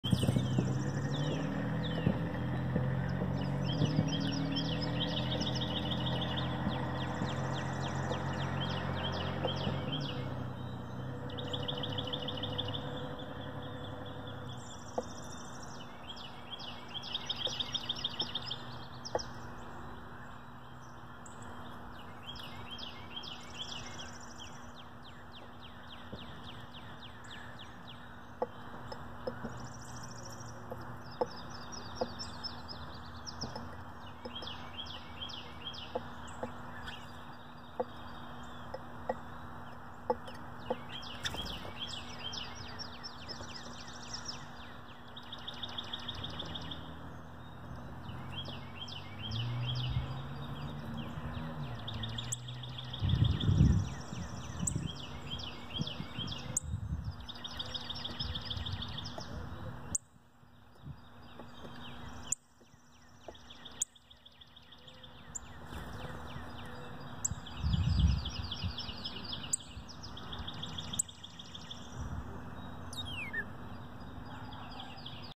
Good Morning Bluebirds
Gathering worms to feed their babies.